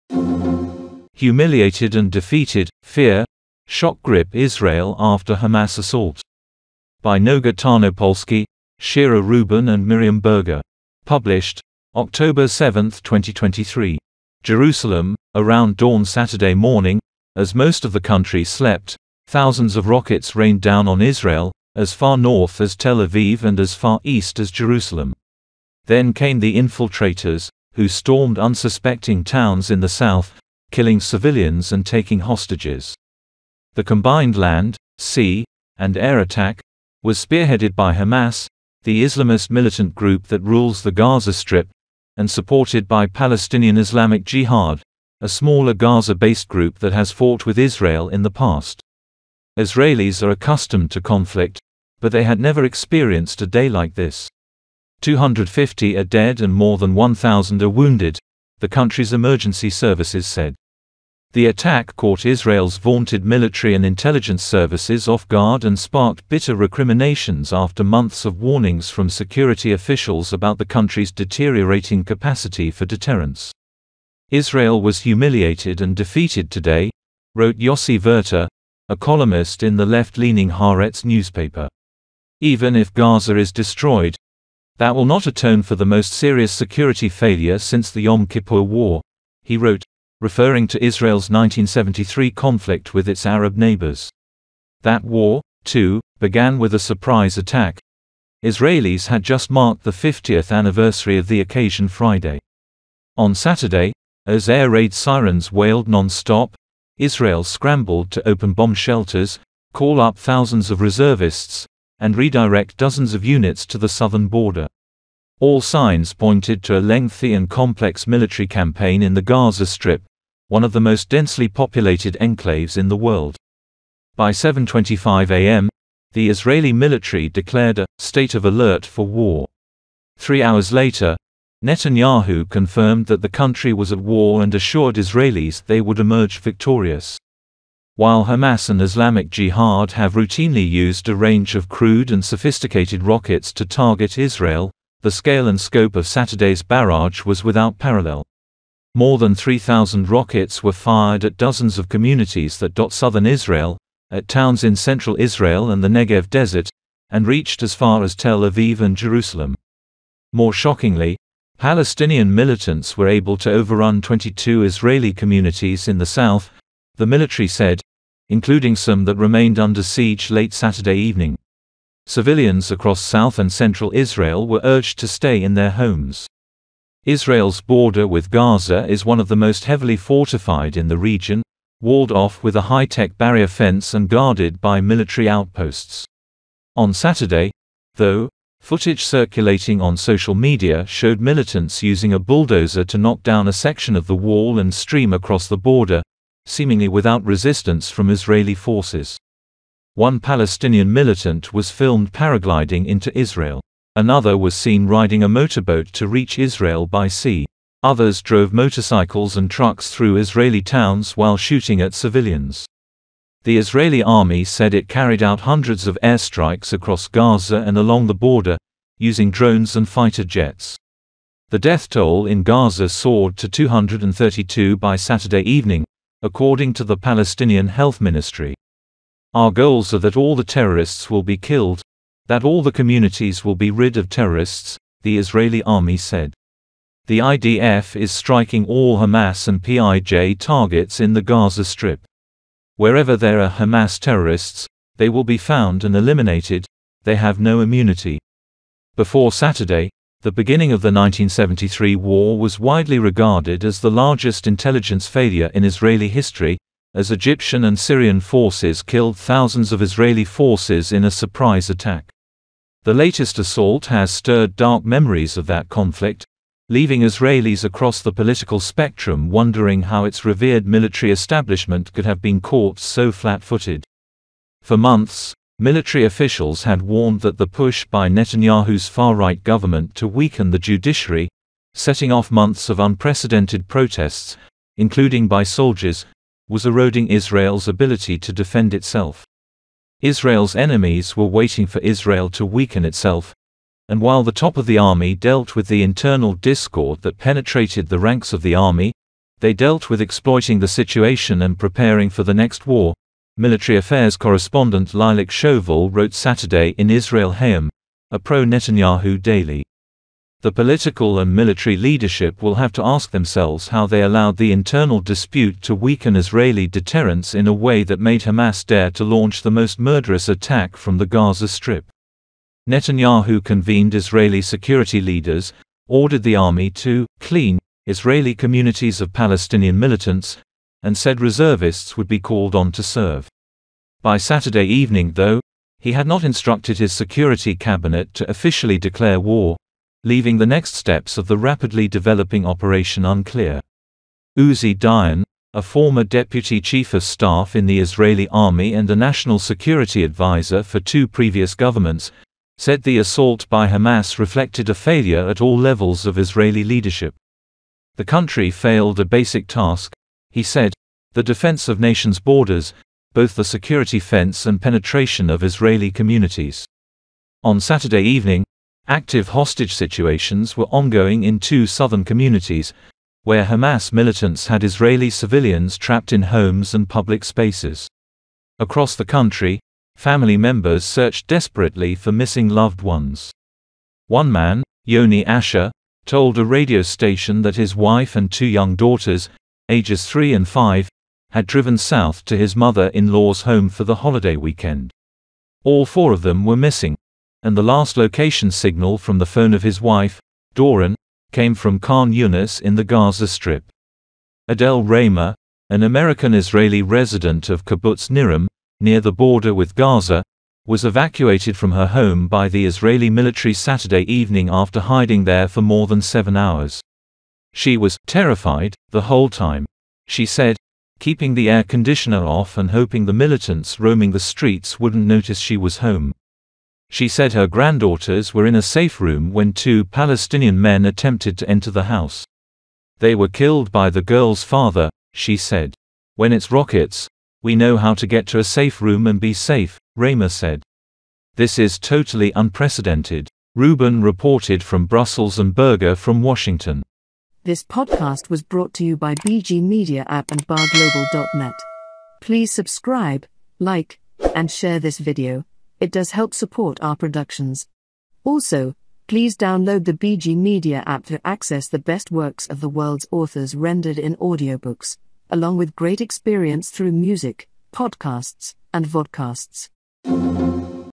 ‘Humiliated and defeated’: Fear, shock grip Israel after Hamas assault By Noga Tarnopolsky
‘Humiliated and defeated’: Fear, shock grip Israel after Hamas assault
By Noga Tarnopolsky, Shira Rubin and Miriam Berger